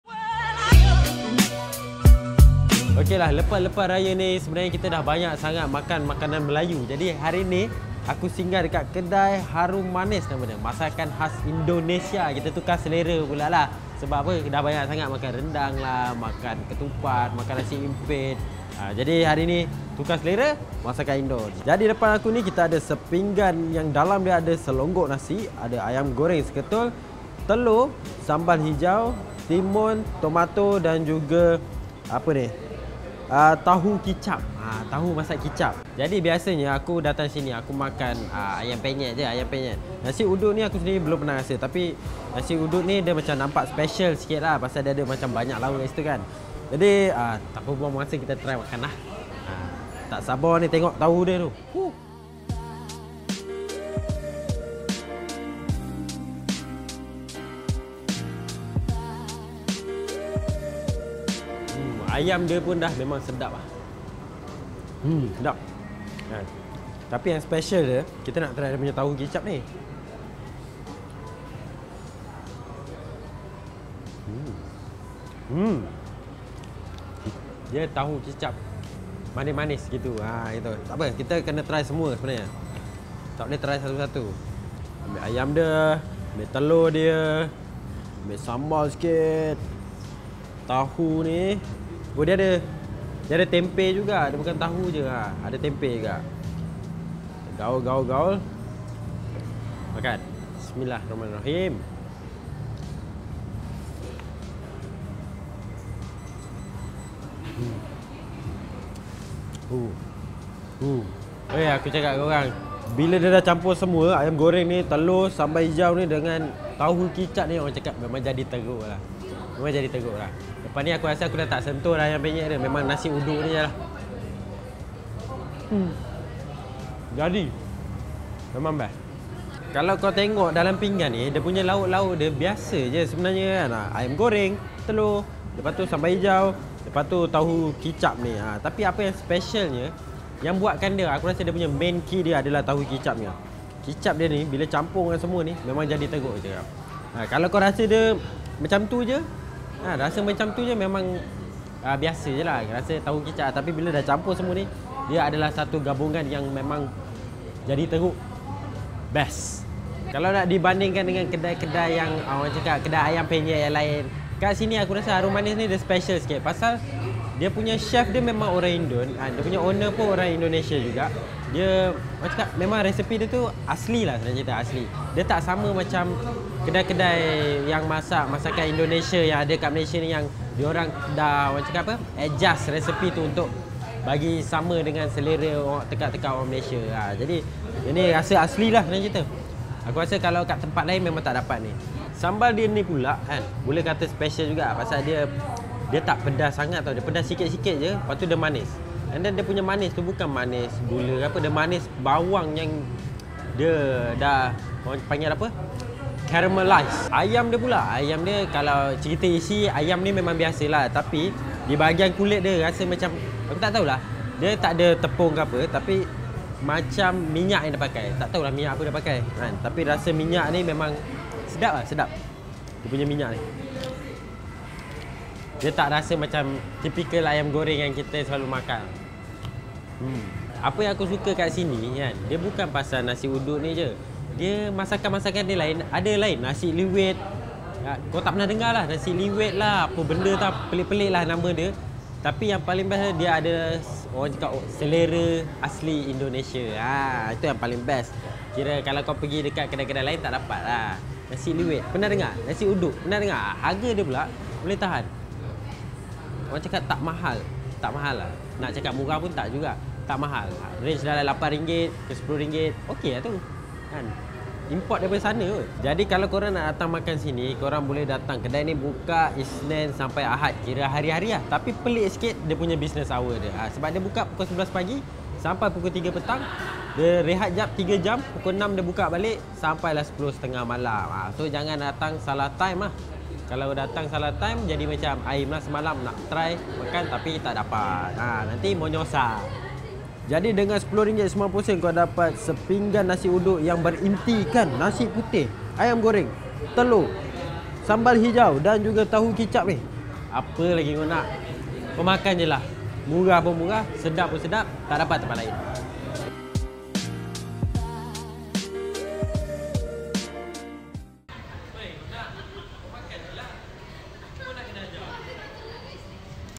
KCHUP MKAN: Nasi Uduk Harum Manis
Sedap gila, original dari negara jiran. Puas hati boi

Address: No 36-G, Jalan Setia Utama AS U13/AS, Setia Alam, 40170 Shah Alam, Selangor

▶Nak buat website? KLIK LINK NI